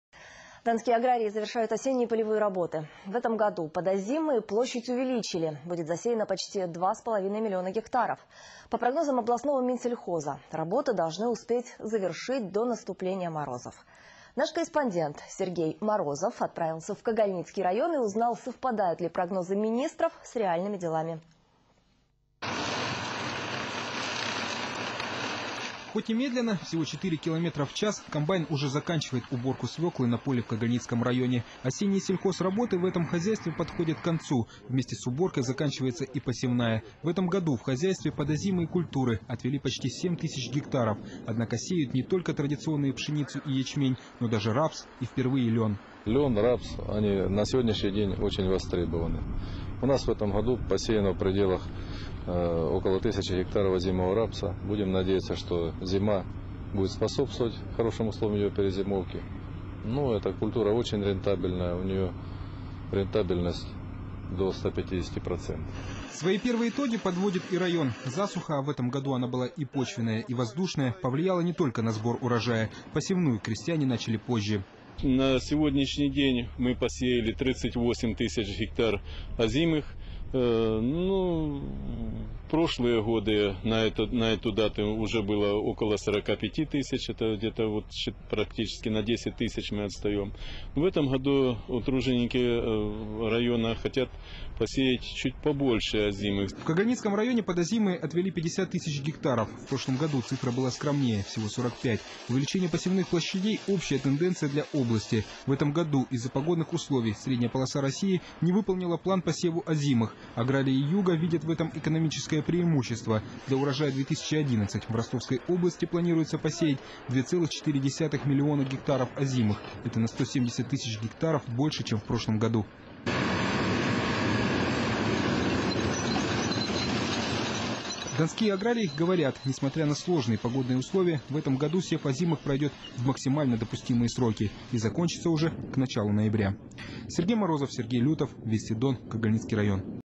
Осенние полевые работы в РО завершаются
Осенние полевые работы в Ростовской области подходят к концу. полевые работы. В 2010 году под озимые площадь увеличили - будет засеяно почти два с половиной миллиона гектаров. По прогнозам областного минсельхоза, работы должны успеть завершить до наступления морозов. DONTR.RU